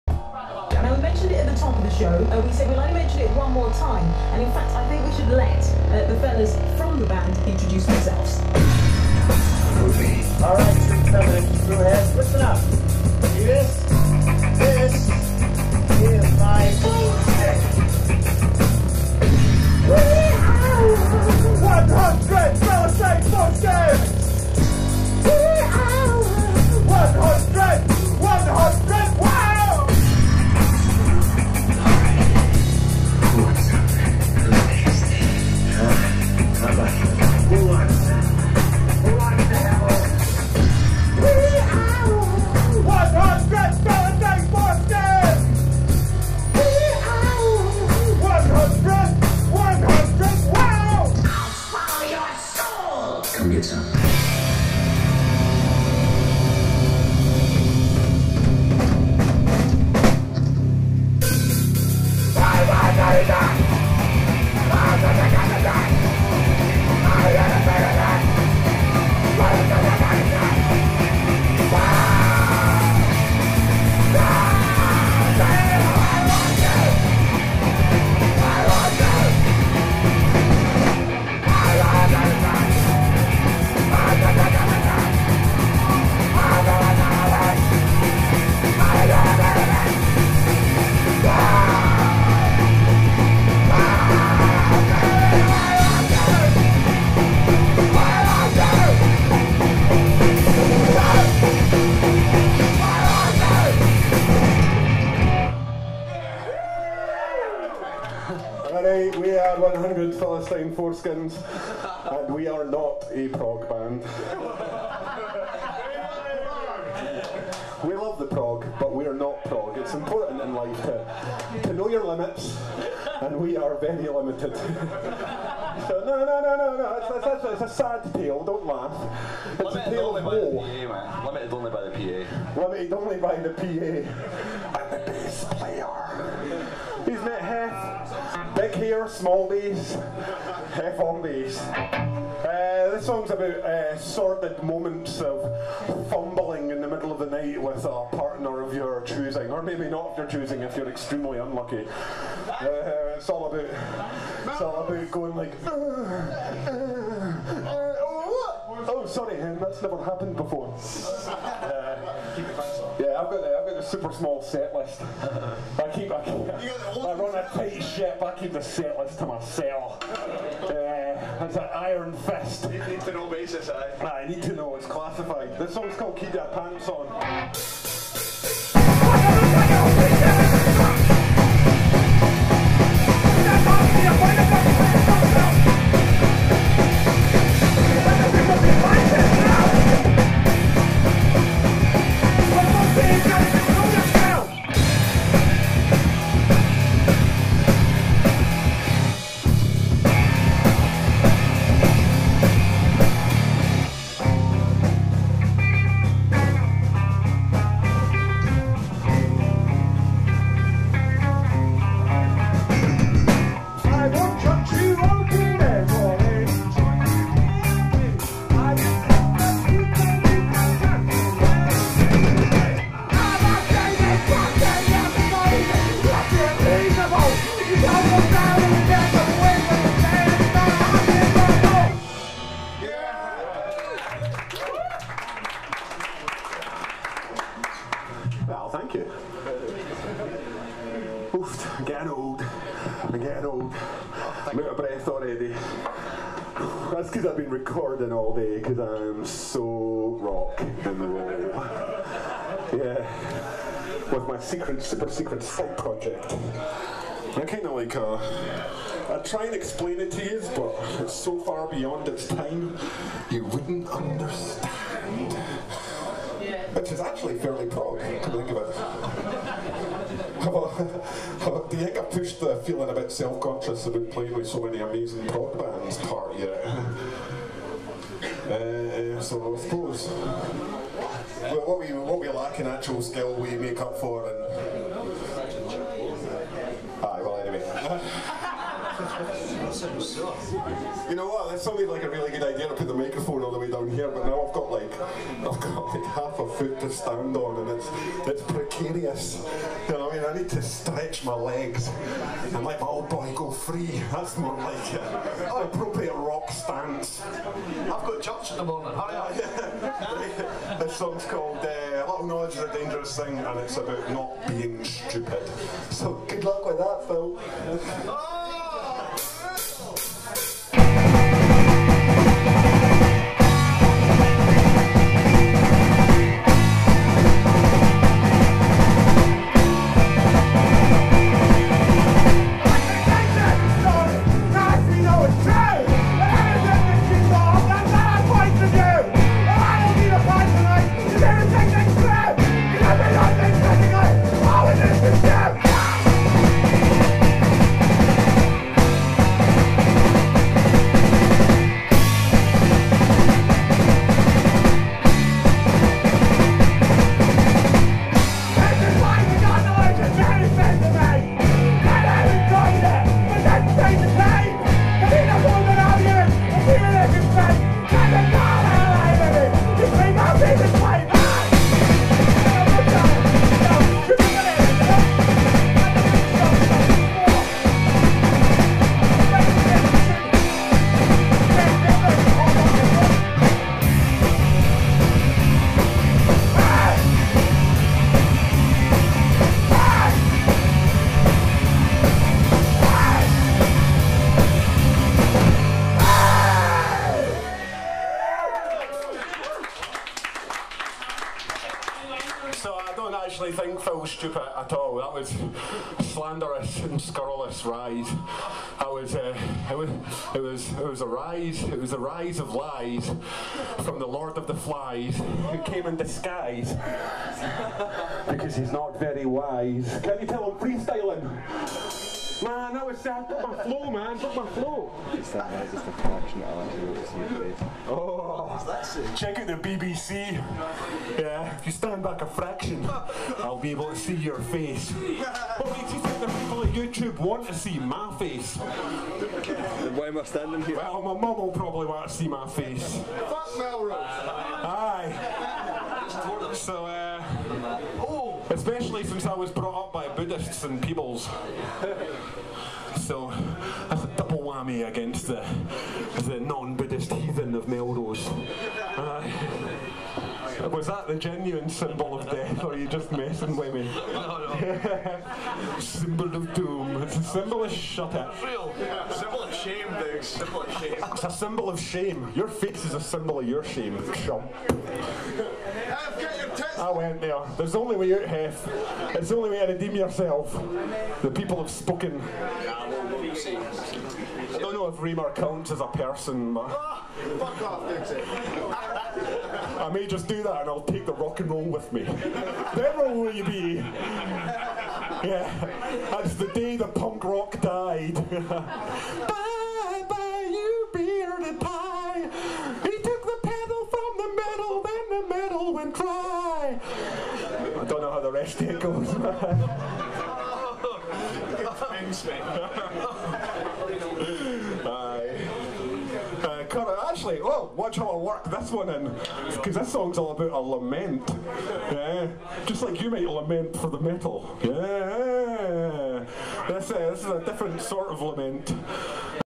100 Philistine Foreskins Live in Edinburgh Part 1
100 PFS play a set in Edinburgh at Henry's